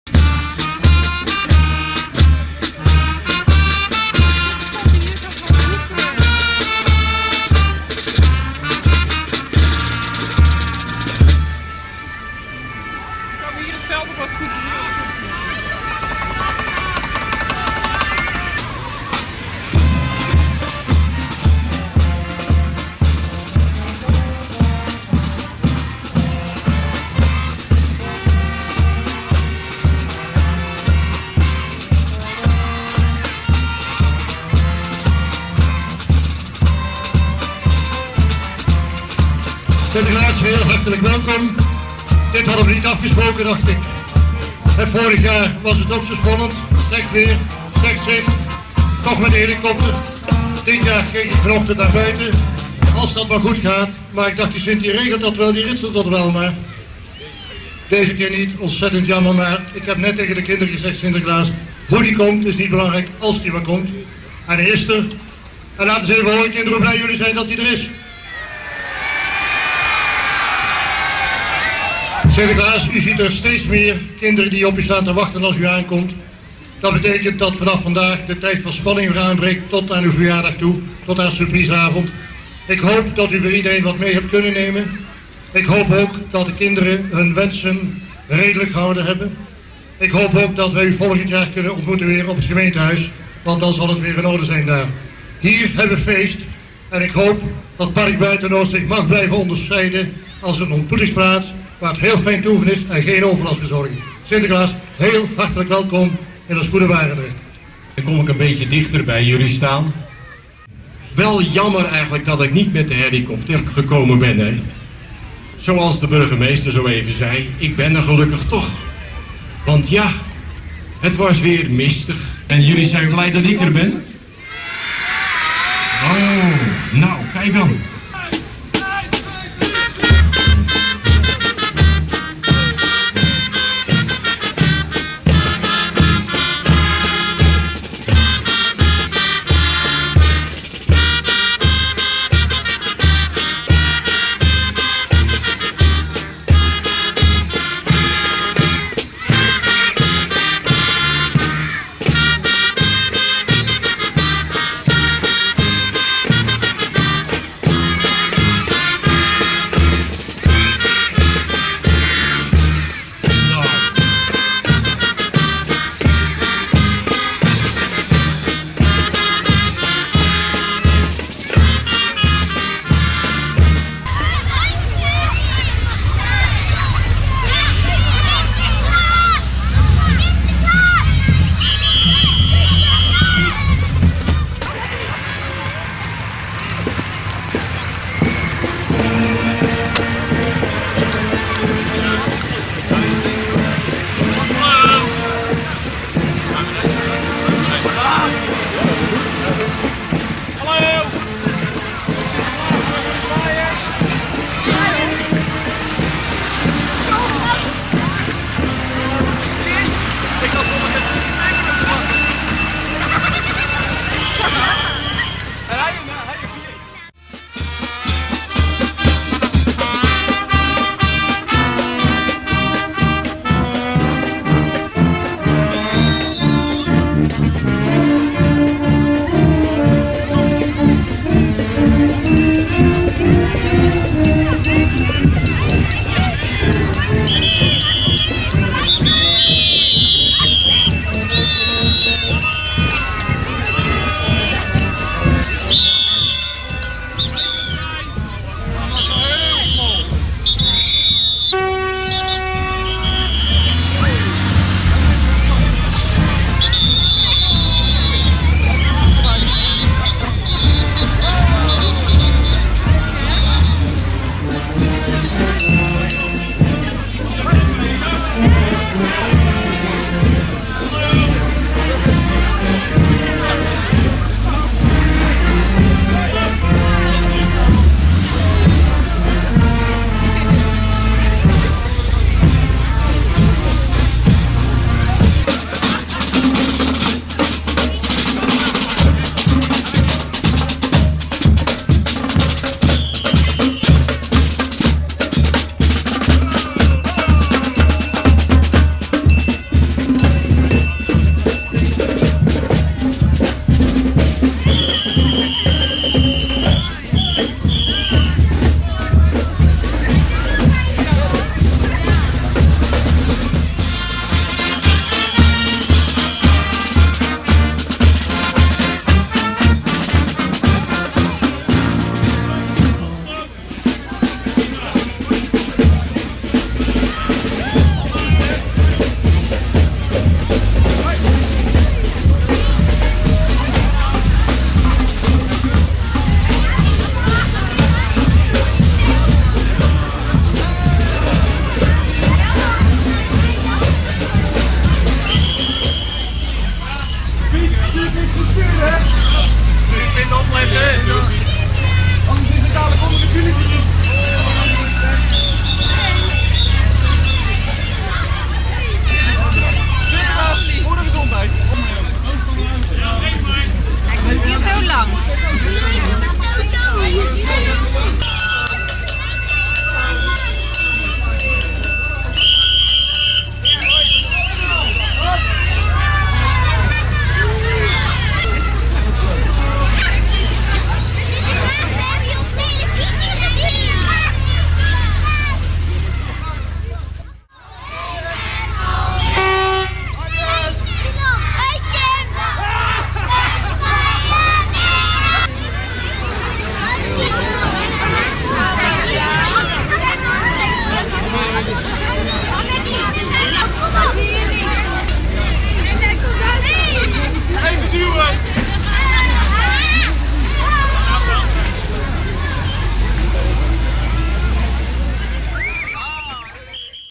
Intocht Barendrecht 2002
Filmverslag over de intocht van Sint Nicolaas in Barendrecht in het jaar 2002.